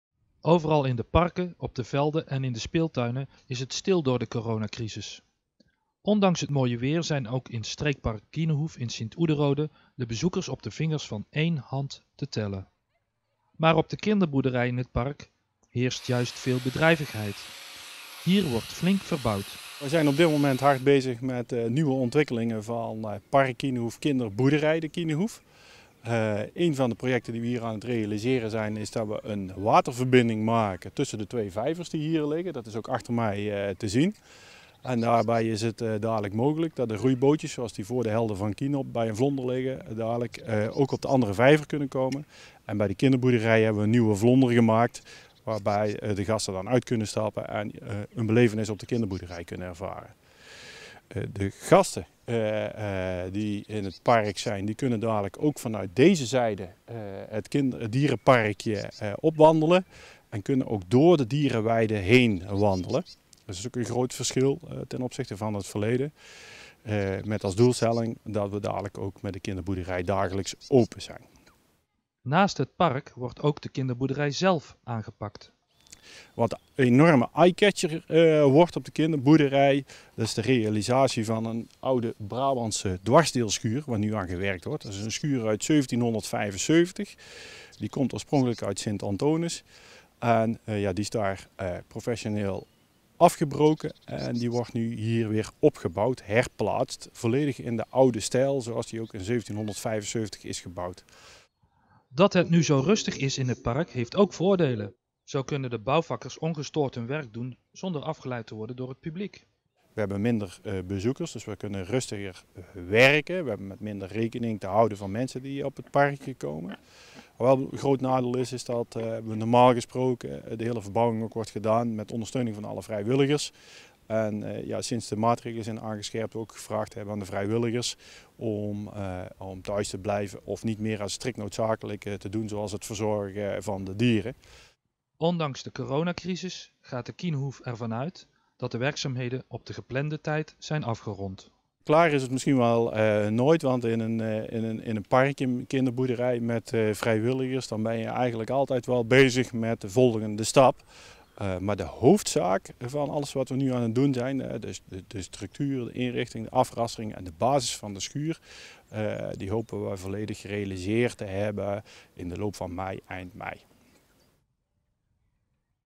Verbouwing Kienehoef
SINT-OEDENRODE - Ondanks de coronacrisis gaat de verbouwing van de Kienehoef in Sint-Oedenrode gewoon door.

De vijvers worden met elkaar verboden zodat de boten straks verder kunnen varen. En de kinderboerderij gaat op de schop.

Collega Rob Dekkers ging er kijken.